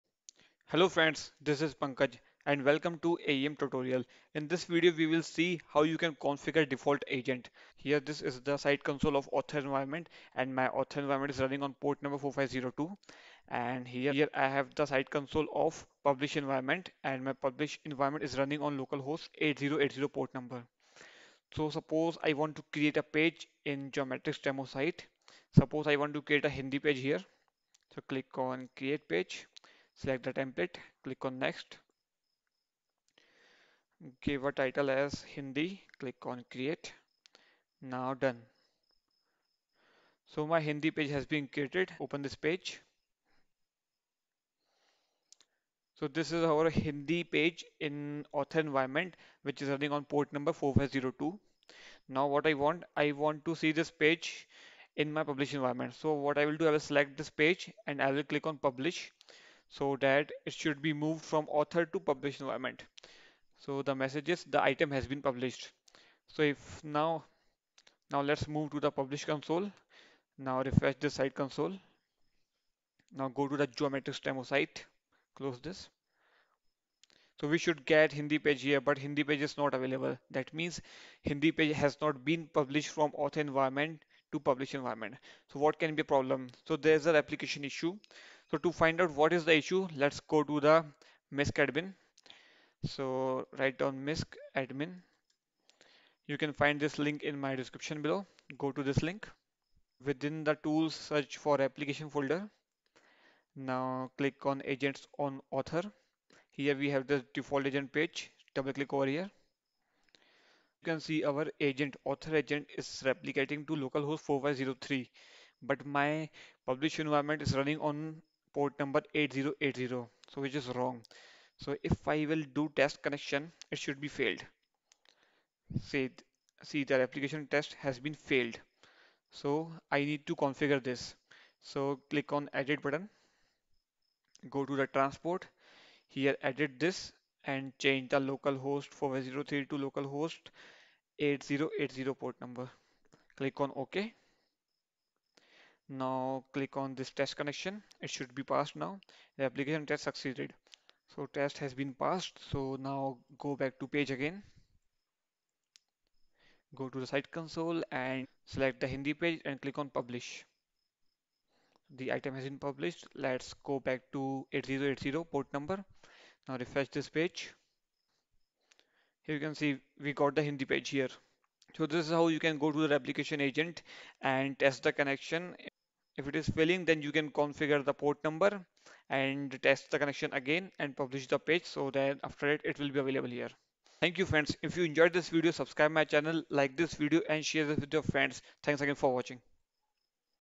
Configure default agent in AEM
Configure default agent in AEM: Moving content from Author to publish environment in AEM is called Replication. If you find your content has not published then in that case you can check your default agent configuration.

Here under the tools, find the replication folder, open the agent on authors, there you will find the default agent page. Open this page, here you can test the connection if it is failing that mean there is a configuration issue.

Edit the configuration and point it to the correct publish environment. Save the  changes and test the connection. If it is passed then you now you an replicate your changes from author to publish environment in AEM.

About : AEM/CQ5 tutorial is a youtube channel which helps you to learn AEM concepts theoretically as well as practically.